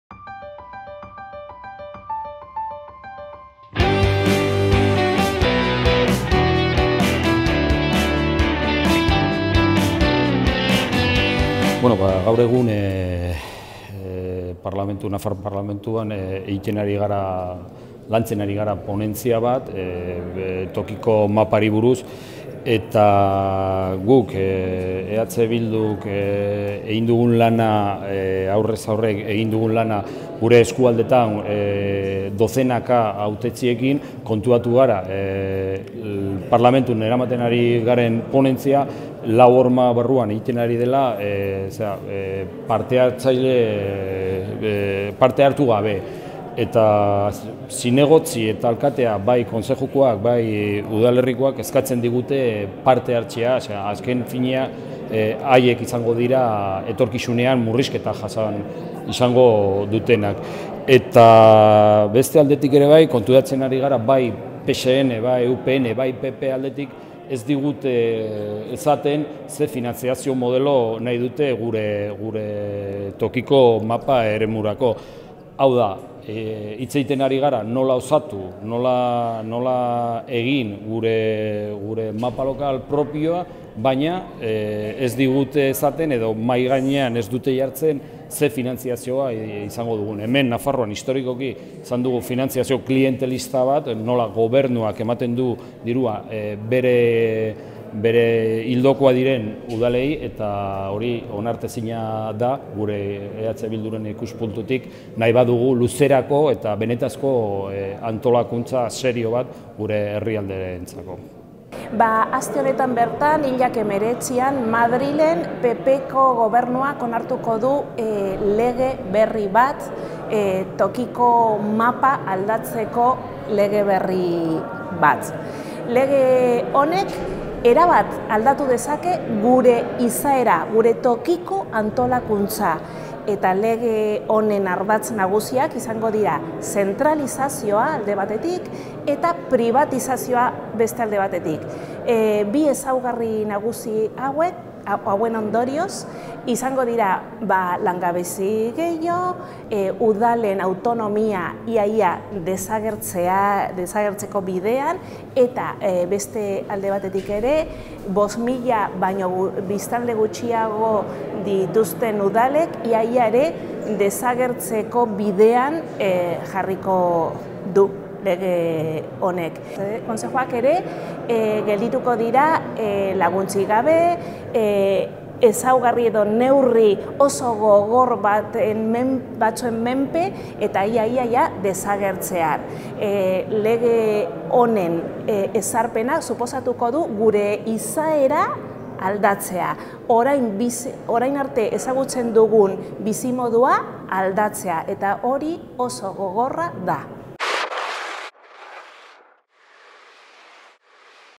Udal Administrazio legeari buruzko batzar zikloa egin du EH Bilduk Nafarroako hautetsiekin
EH Bilduk eskualdez eskualde batzar zikloa burutu du Udal Administrazio legeari buruzko hausnarketa hautetsiekin konpartitzeko. Atzo, abenduaren 17an, burutu zen azkena Iruñean.